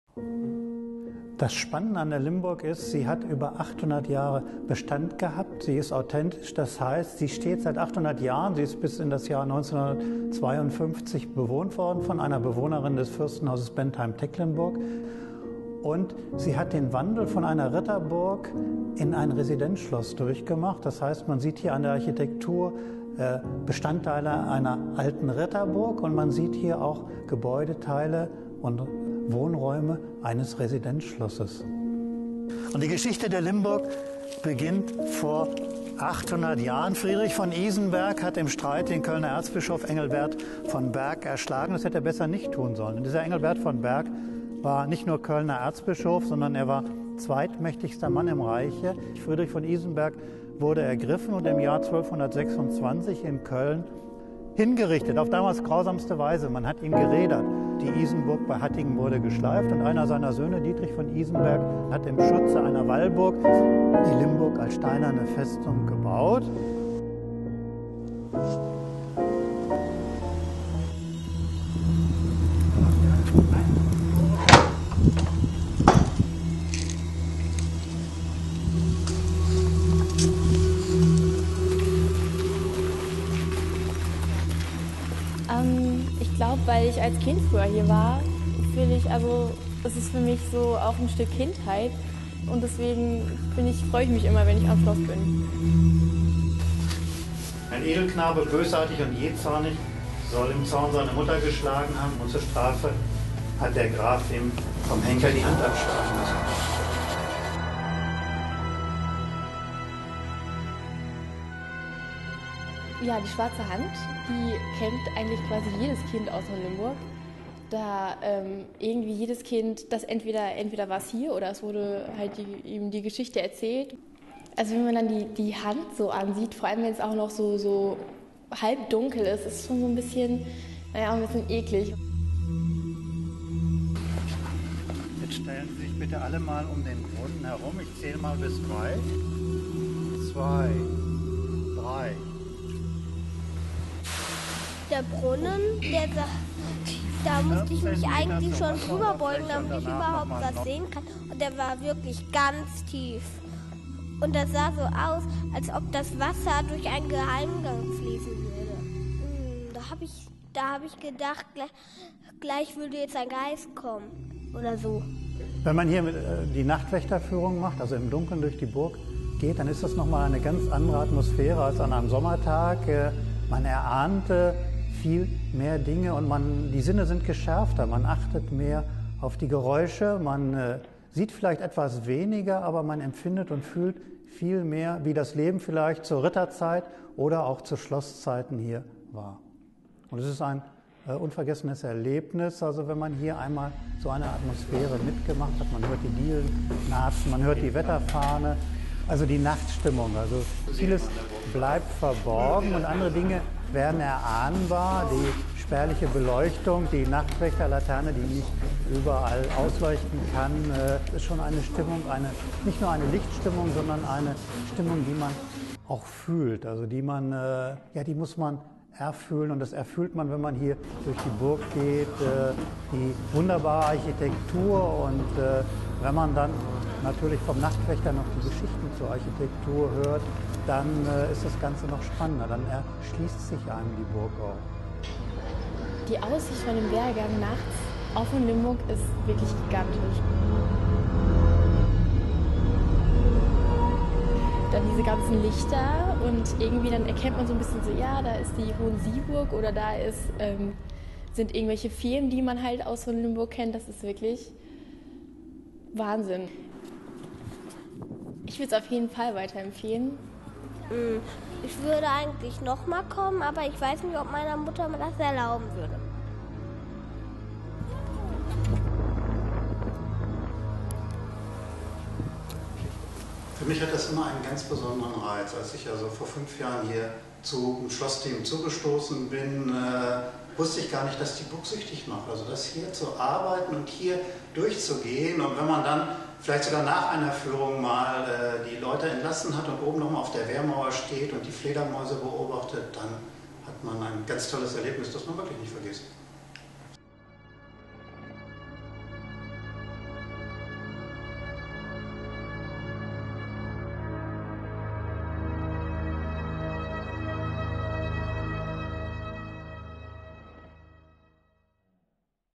Nachtwächterführung Schloss Hohenlimburg
Die Besucher werden vom Nachtwächter persönlich am Schlosstor empfangen. Sie erkunden dann bei Kerzenlicht die Räumlichkeiten des Schlosses und lauschen den Sagen, Mythen und wahren Geschichten rund ums Schloss Hohenlimburg. Was hat es wirklich mit der Schwarzen Hand auf sich? Wer war die Weiße Frau? Was befindet sich in dem Sarg?